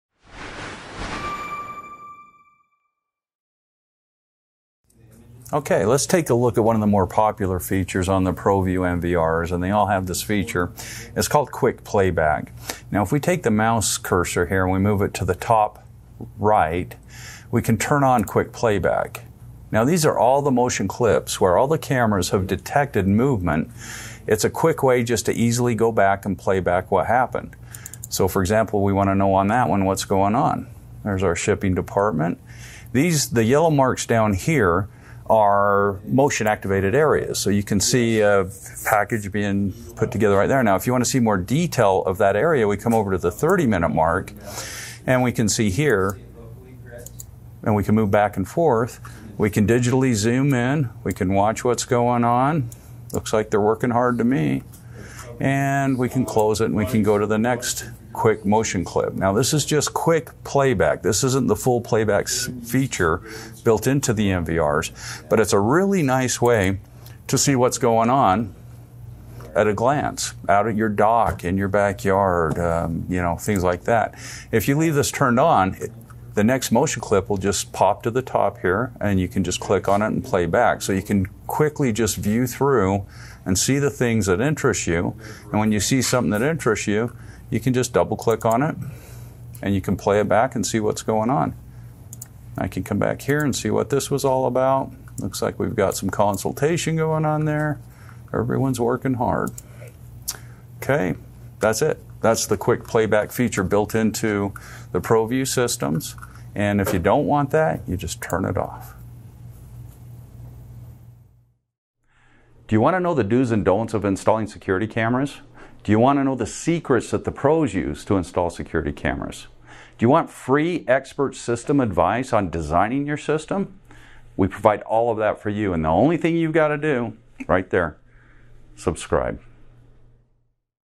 ProVue Video Surveillance System - Quick Playback Features
See the ProVue software in action: enabling and disabling quick playback and viewing of quick playback motion activated clips. All of this is done with the simple click of a mouse.

SECURITY CAMERAS MADE SIMPLE Learn everything you need to install a security camera system & get it right the first time.